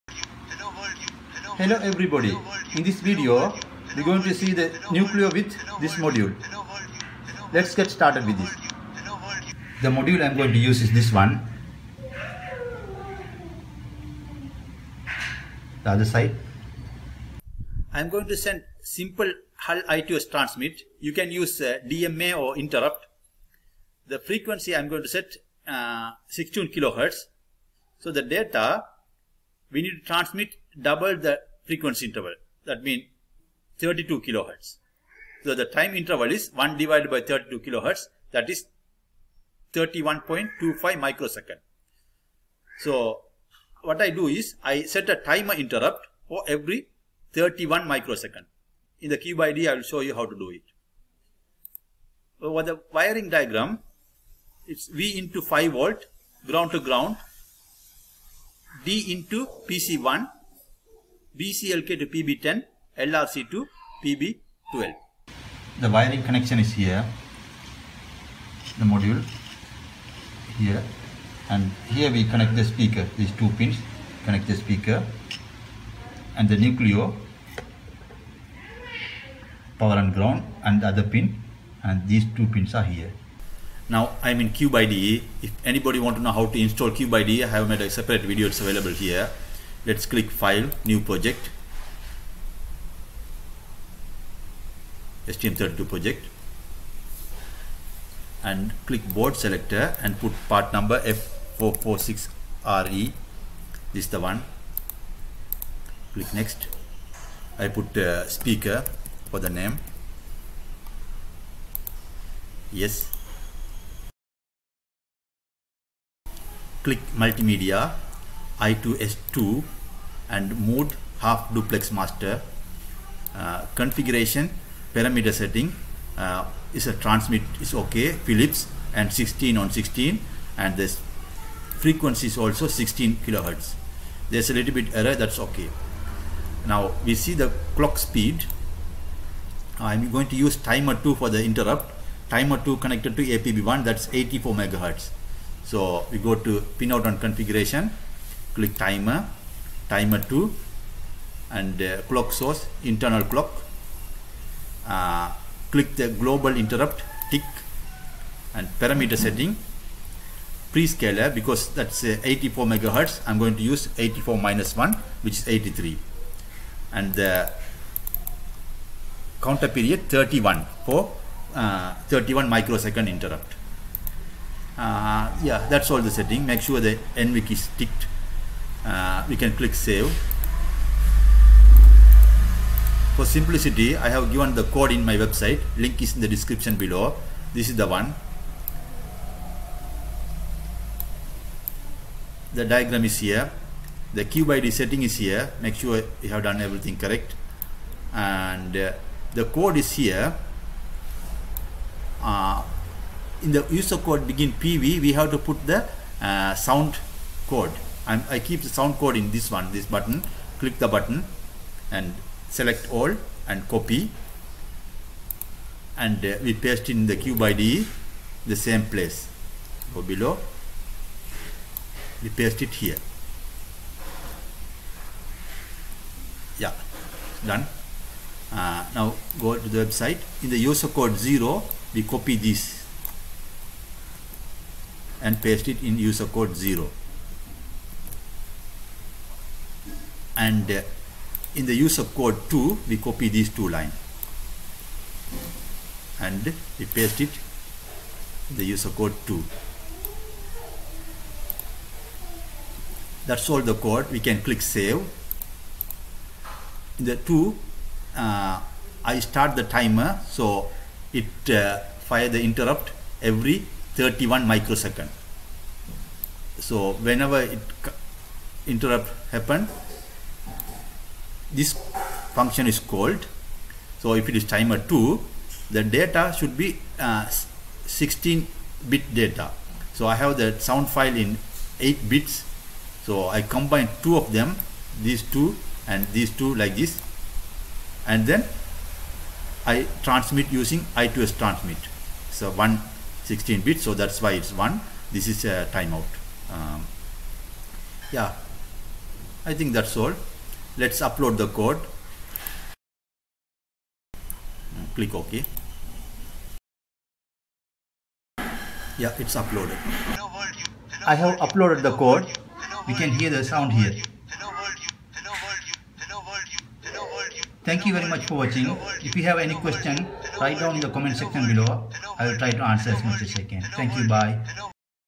117. STM32CubeIDE I2S. MAX98357A with STM32 F446RE Nucleo
Play sound from memory to speaker using STM32